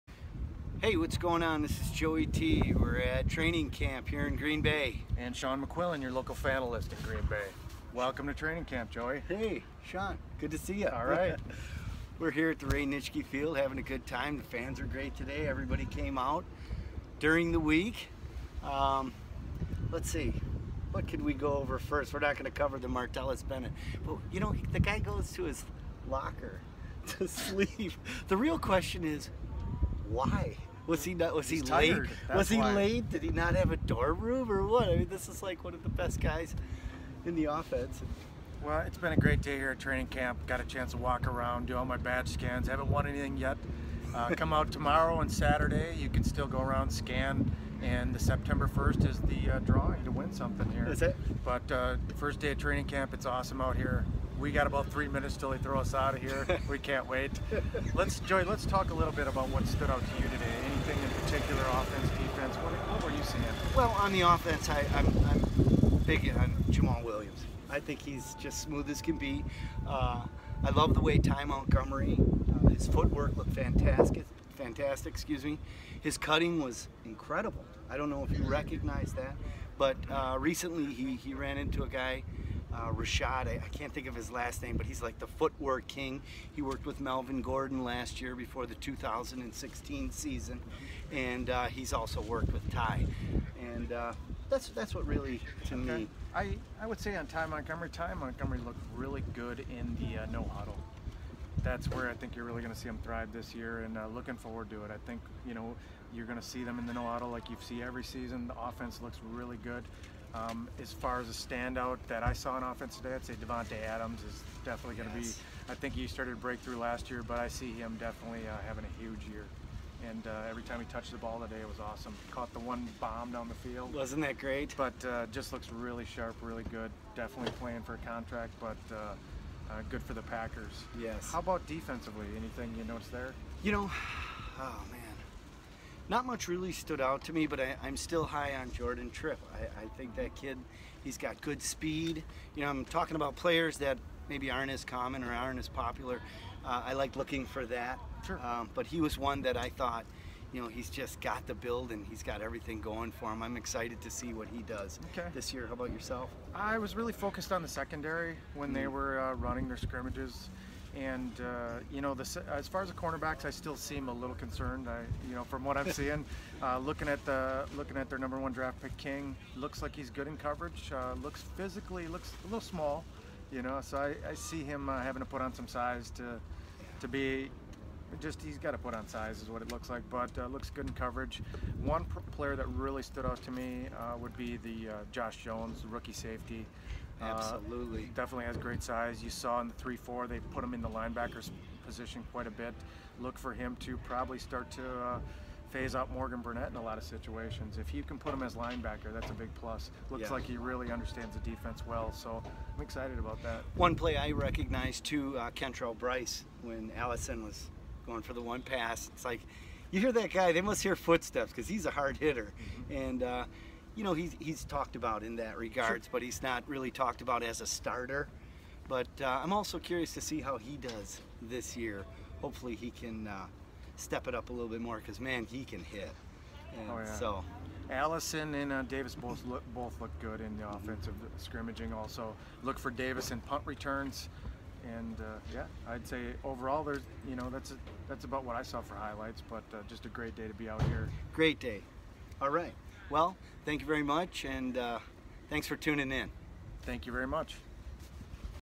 DHPF Tailgate Talk Training Camp July 27,2017
TAILGATE TALK
Joey T and Sean "The Fanalyst" McQuillan with a quick rundown following day one of Training Camp inside Ray Nitschke Field.